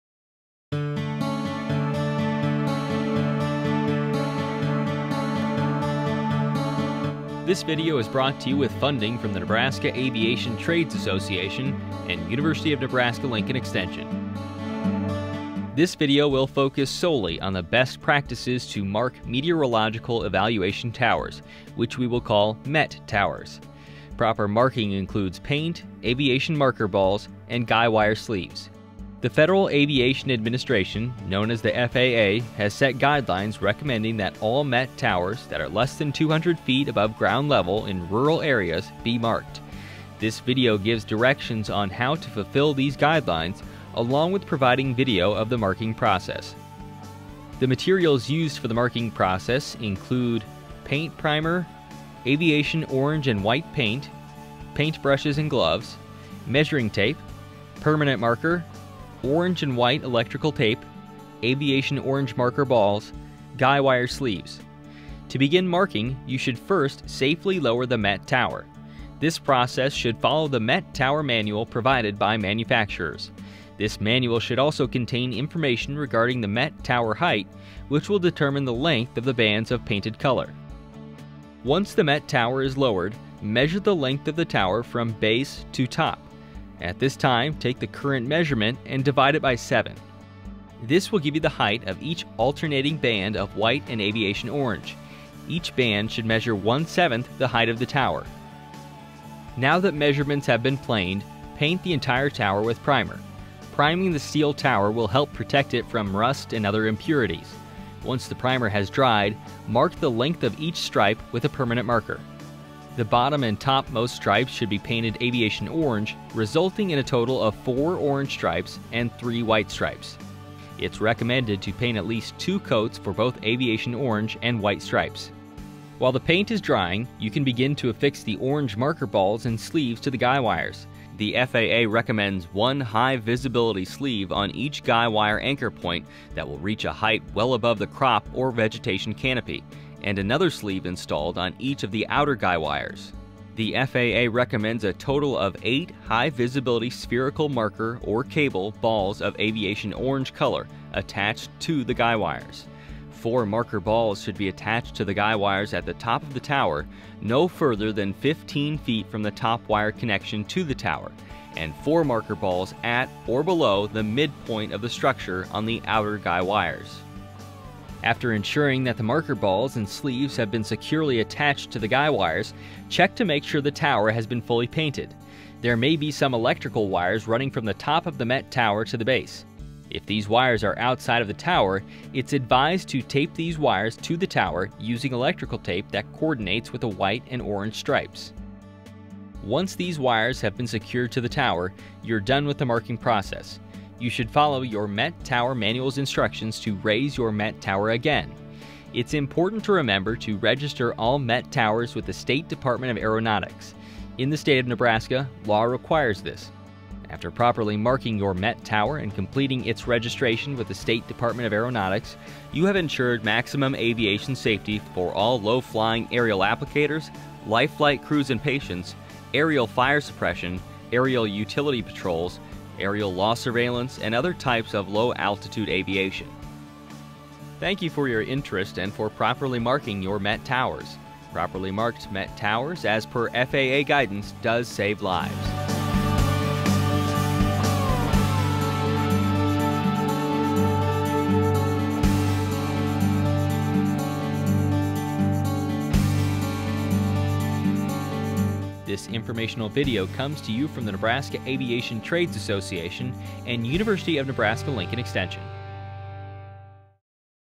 Proper Marking of Wind Measurement Towers for Aviation Safety
Safety Markings for Wind Measurement Towers (Meteorological Evaluation Towers MET).  MET towers are erected to measure wind speed, direction and duration for siting of large wind turbines.  Due to their height of less than 200 ft. they are not required to be lighted.  The Federal Aviation Administration FAA has issued guidance on how best to mark them  to improve safety of low flying aircraft.  Marking includes orange and white paint, orange marker balls on the guy wires, and high visibility sleeves.